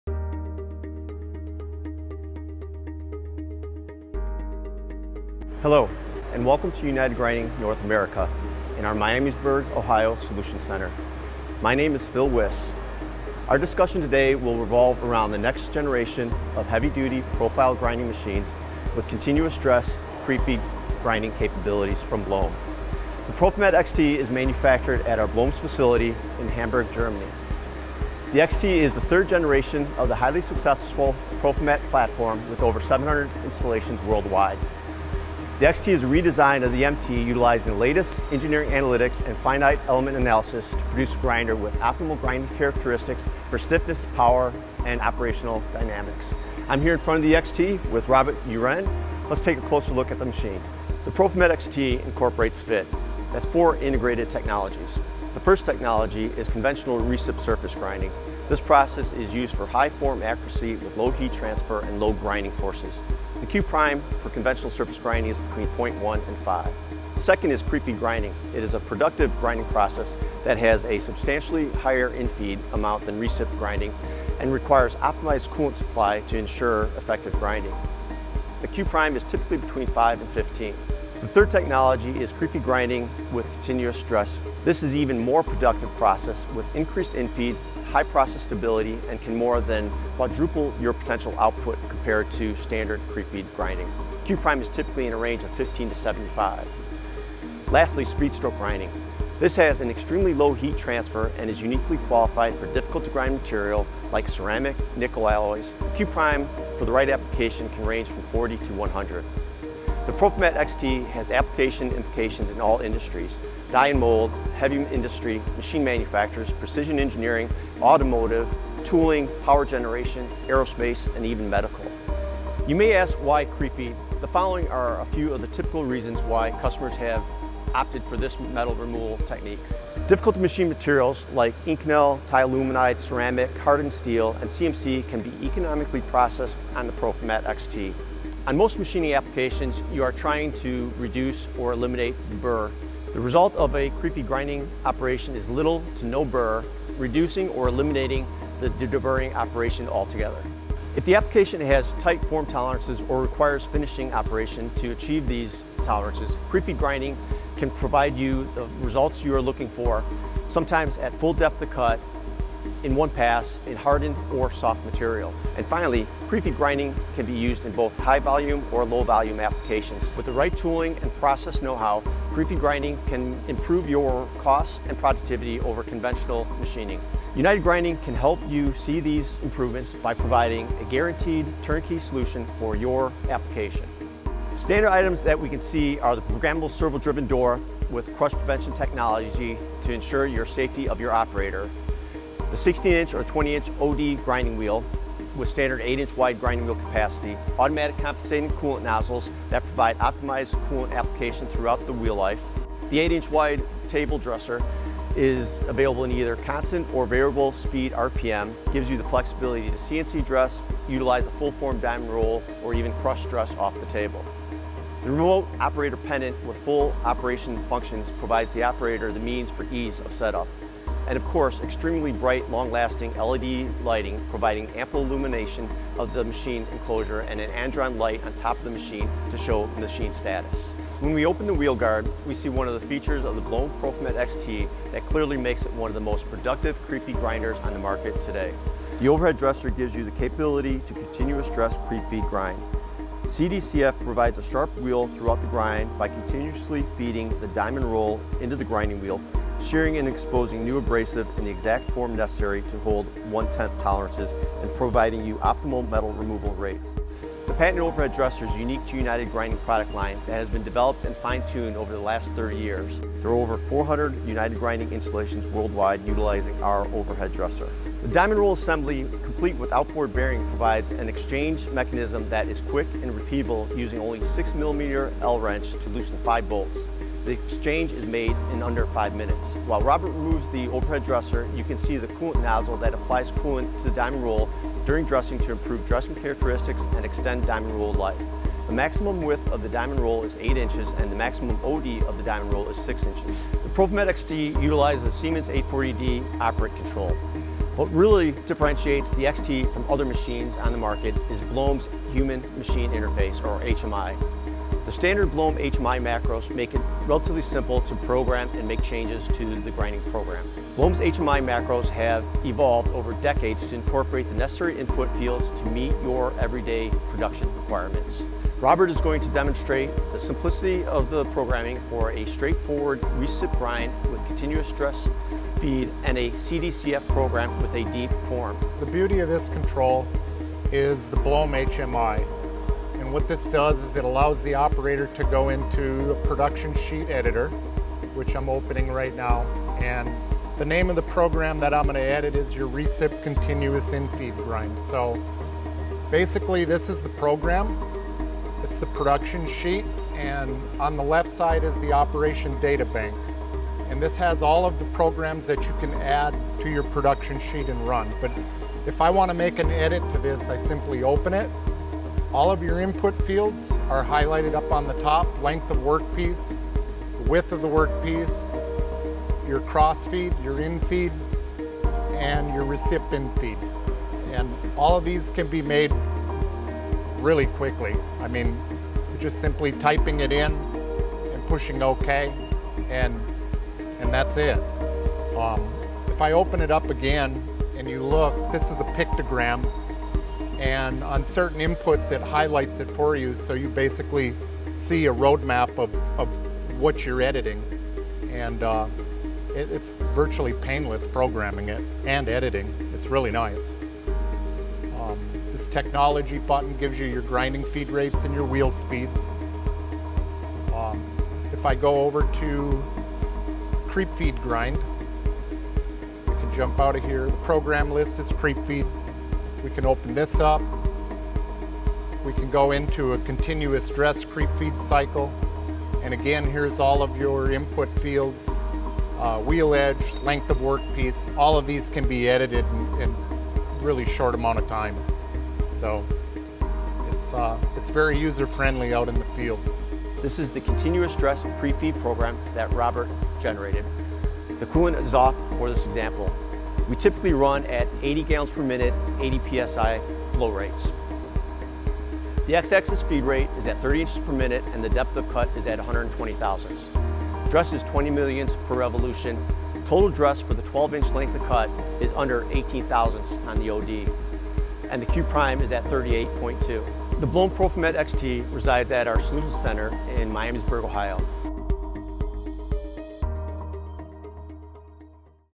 BLOHM PROFIMAT XT Machine Overview by UNITED GRINDING North America
In this video, Phil Wiss delivers an overview of BLÖHM's heavy duty profile grinding machine, the PROFIMAT XT. Join Phil has he discusses key features and benefits of the machine and provides a high-level review of the various grinding processes the machine can support.